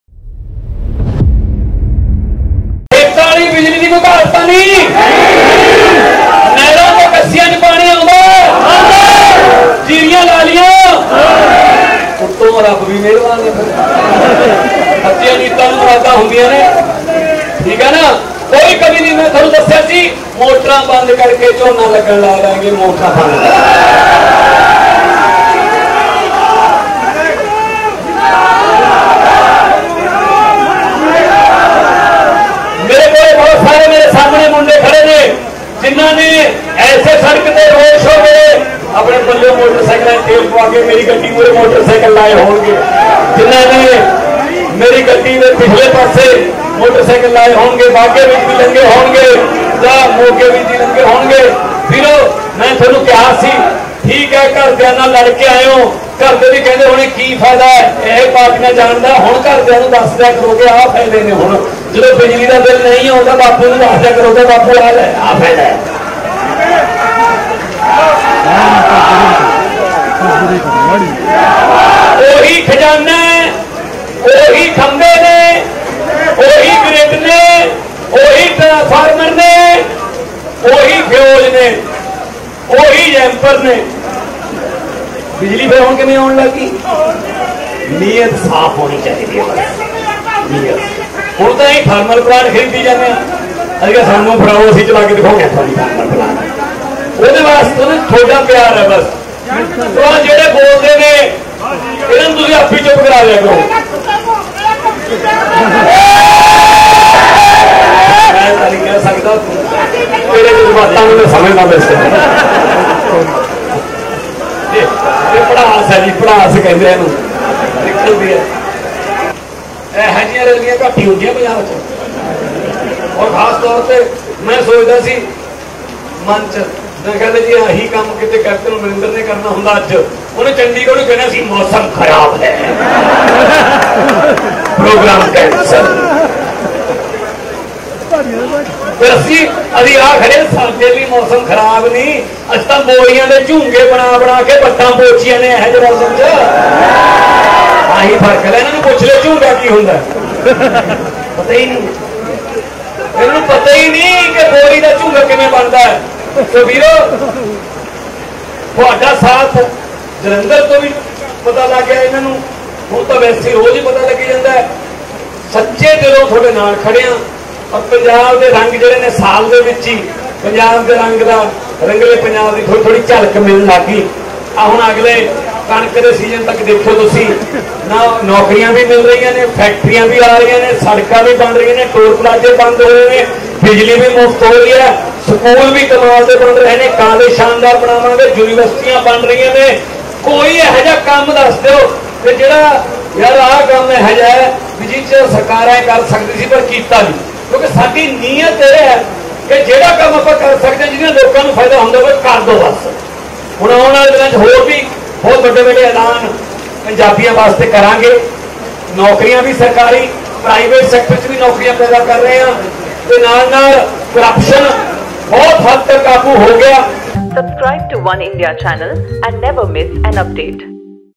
CM Mann ਦੀ ਧਾਕੜ ਸਪੀਚ ਵਿਰੋਧਿਆਂ ਨੂੰ ਦਿੱਤੇ ਠੋਕ-ਠੋਕ ਜਵਾਬ | Cm Bhagwant Mann | OneIndia Punjabi
CM ਮਾਨ ਦੀ ਧਾਕੜ ਸਪੀਚ ਵਿਰੋਧਿਆਂ ਨੂੰ ਦਿੱਤੇ ਠੋਕ-ਠੋਕ ਜਵਾਬ, 'ਕੈਪਟਨ ਨੇ ਕਹਿਣਾ ਸੀ ਮੌਸਮ ਖਰਾਬ, ਰੈਲ਼ੀ ਕੈਂਸਲ' |
CM Mann's blunt response to opponents of the speech.
                ~PR.182~